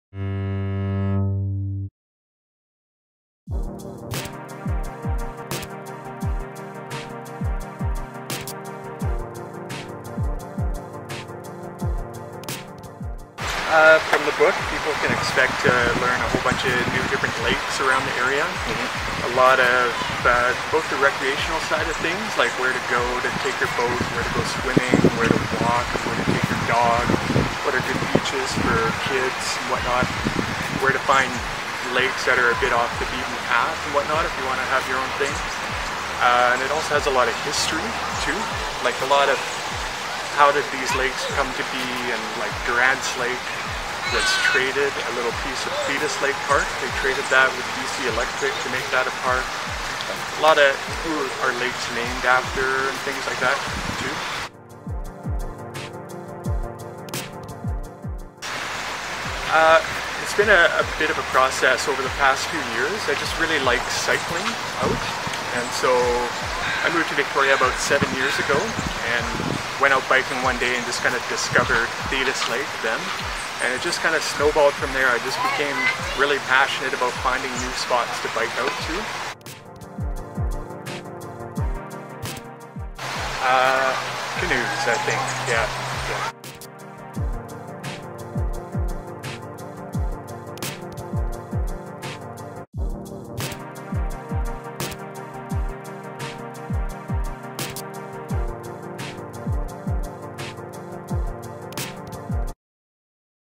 Three Questions - Adam Ungstad (Secret Lakes of Southern Vancouver Island)
This week in "Three Questions With ..." The Martlet meets up with Adam Ungstad, author of Secret Lakes of Southern Vancouver Island. His book provides a focused exploration of local lakes and acts as a great guide for people looking to stroll on a beach, catch some fish or slip into a sunken hideaway.

You can find his book at a plethora of local bookstores